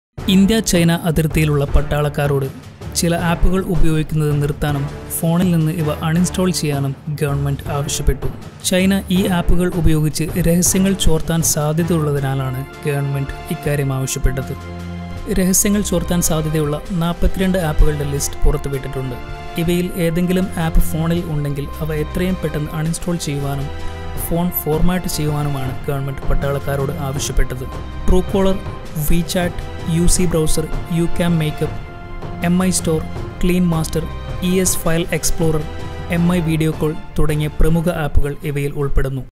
Apps Banned by Indian Army | Chayakkada Malayalam News
New list of apps released by Indian Army for their soldiers at Chinsese borders to uninstall for possible Chinese spyware.